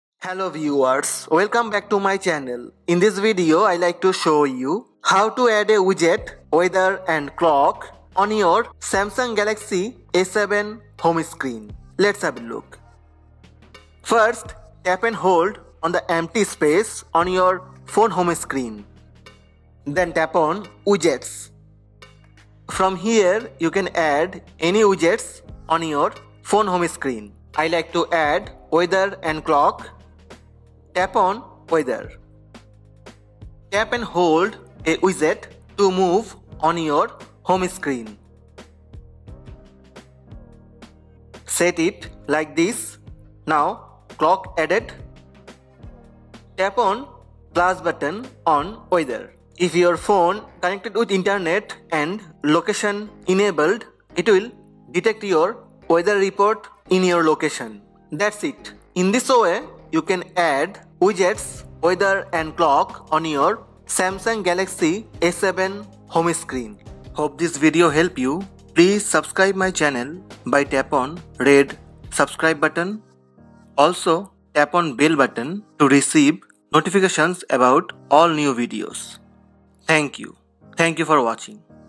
How to Add a Widget (Weather & Clock) on Home Screen - Samsung Galaxy A7 (2018)
Samsung Galaxy A7 (2018) - How to Add a Widget (Weather & Clock) on Home Screen.